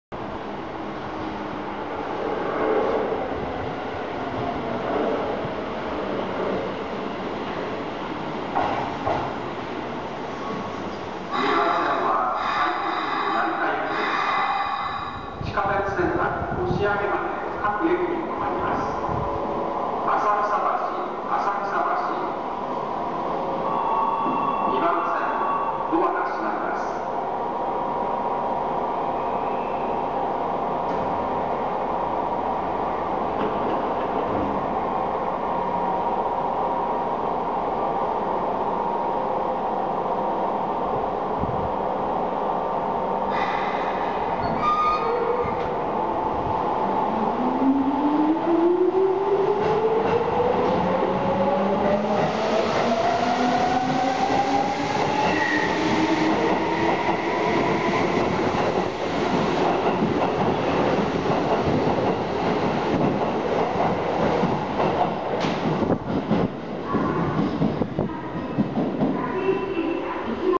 都営地下鉄 浅草線 浅草橋駅 快特 京成 成田行き 3600形 到着・発車
都営地下鉄 浅草線 浅草橋駅で快特 京成 成田行き3600形電車の到着・発車を